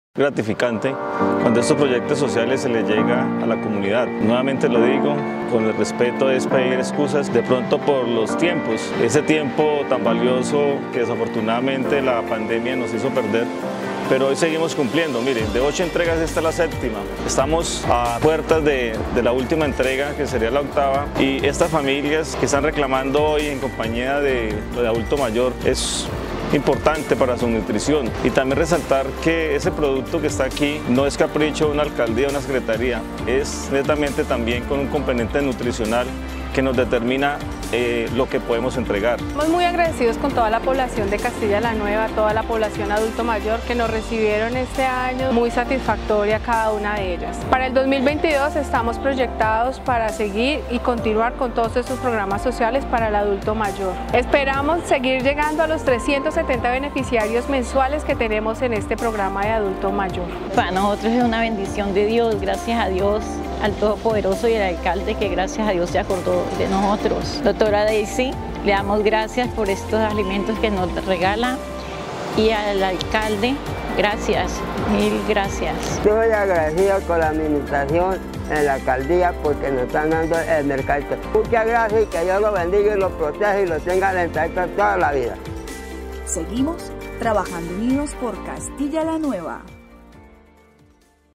MERCADOS ADULTO MAYOR CASTILLA LA NUEVA
AdultoMayor | La Administración Municipal en cabeza del Alcalde Fernando Amézquita, a través de la Secretaría de Salud Pública y Protección Social realizaron la séptima entrega de este año de complementos nutricionales y kits de aseo para los adultos mayores del Municipio, nos complace ver los rostros de satisfacción y alegría de este grupo poblacional, y nos motiva a seguir trabajando por este tipo de programas sociales para Castilla la Nueva. ¡Seguimos! TrabajandoUnidos por Castilla la Nueva.